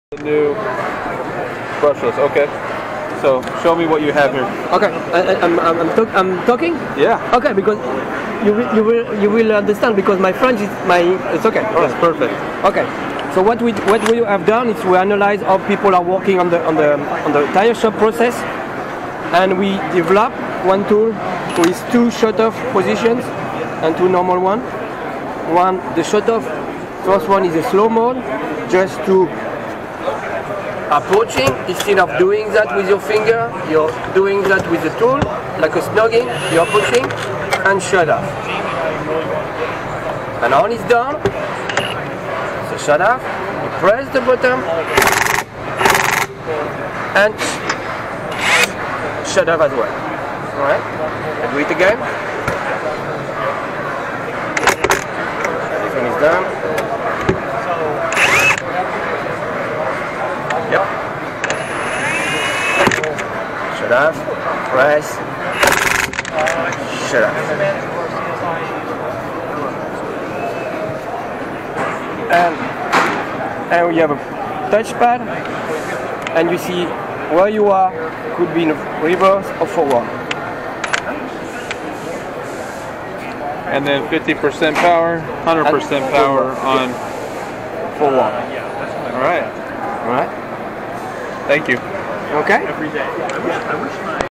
Chicago Pneumatic CP8849K-4AH 20-Volt 1/2-Inch 4.0-Amp Cordless Impact Wrench Short DEMO
Check out the Newly Released Chicago Pneumatic tool and its functionality.